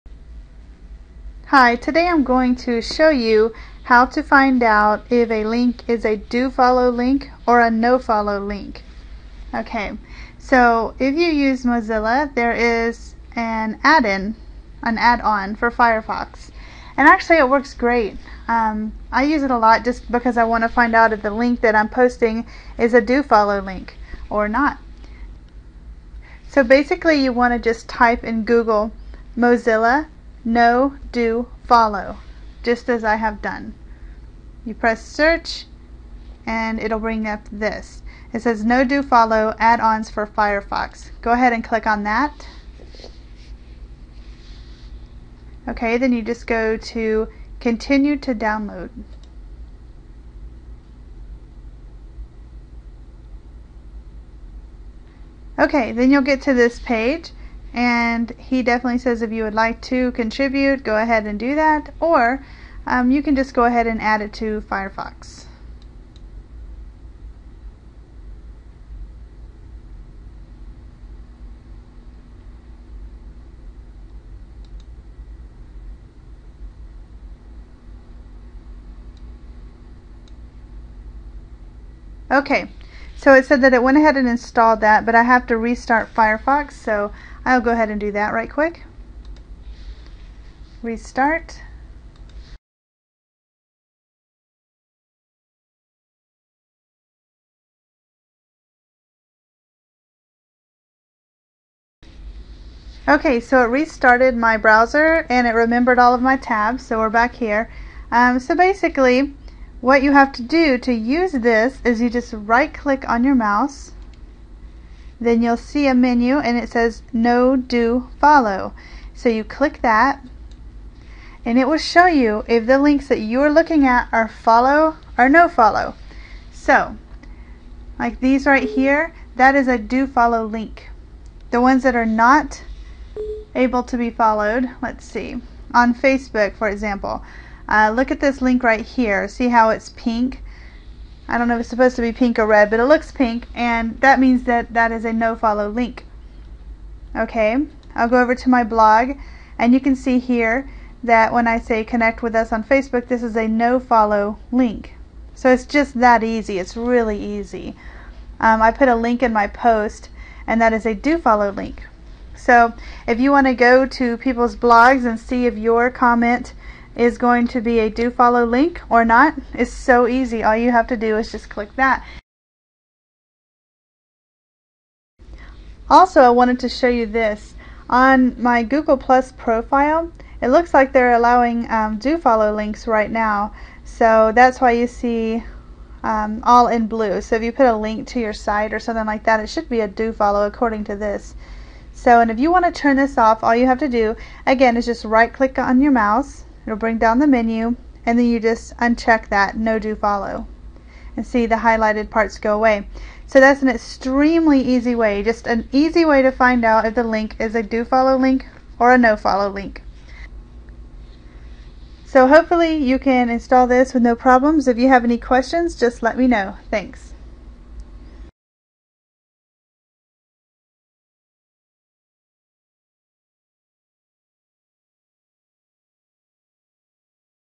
Free add-on shows if a link is "no" or "do" follow - Firefox nodofollow
How do you know if the link you are looking at is "no follow" or "do follow". I'll show you a free add-on for  Mozilla Firefox that will show you what type of link you are looking at - and fast! This is super easy.